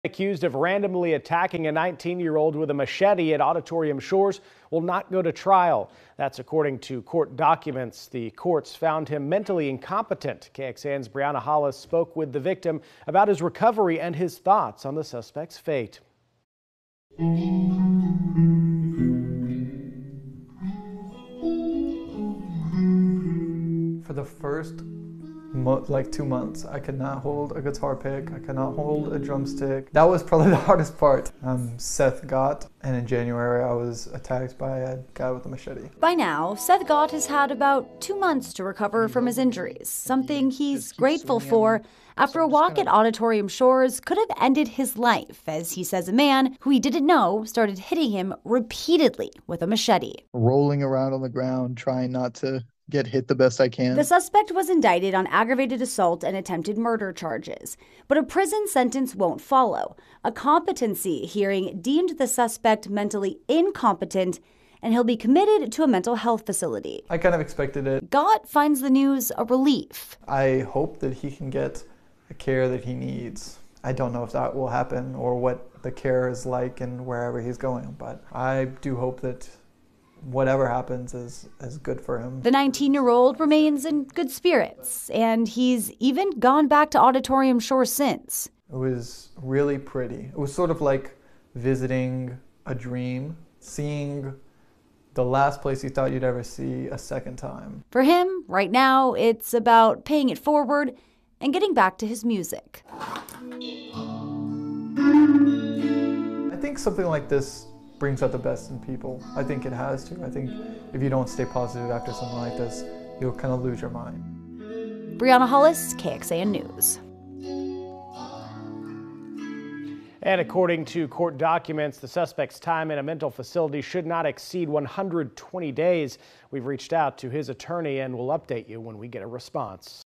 “Hope he gets the care he needs,” machete attack victim on suspect’s mental incompetency finding
For 19-year-old Seth Gott, the hardest thing about his recovery from a machete attack at Auditorium Shores was not being able to play music for a month.